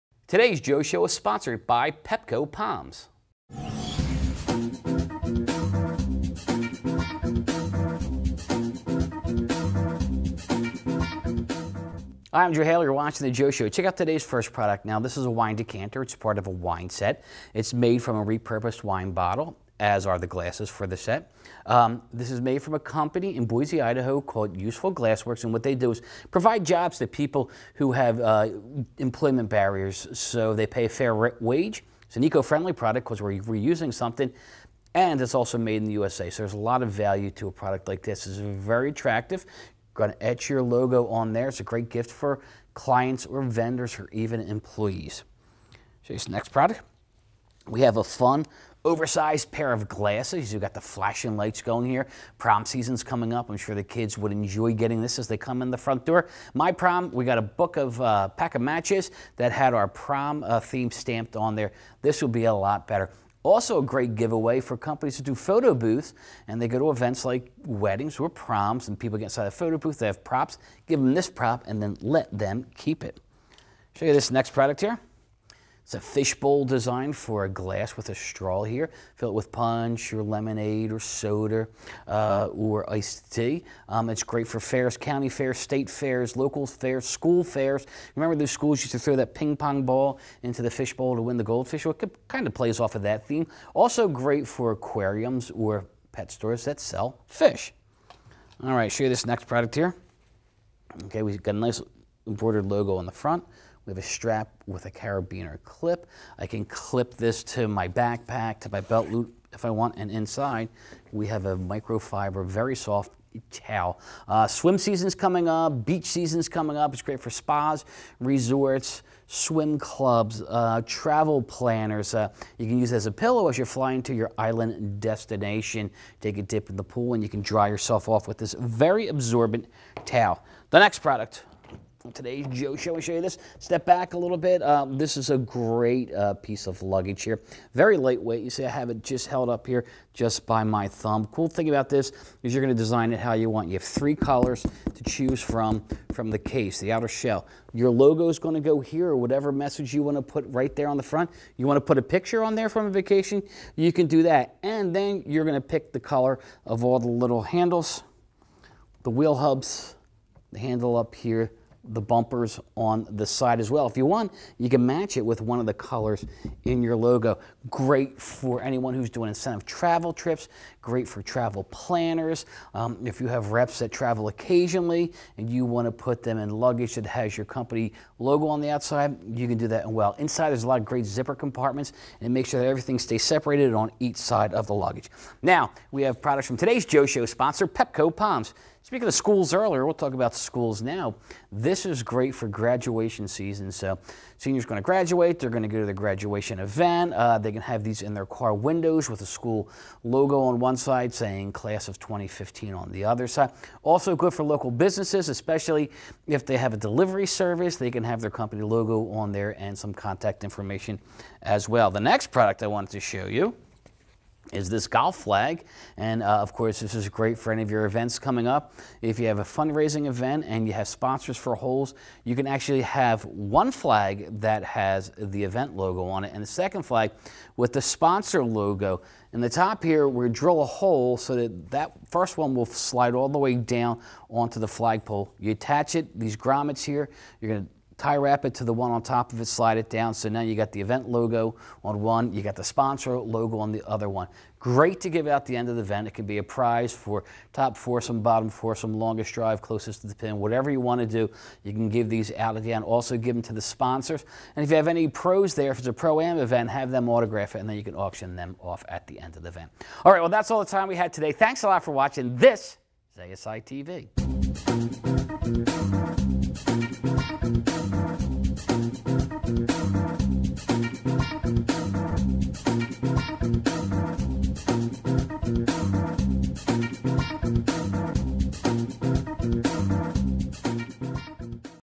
Cheers to New Products
Joe Haley, host of the Joe Show, features great new products in this week's installment.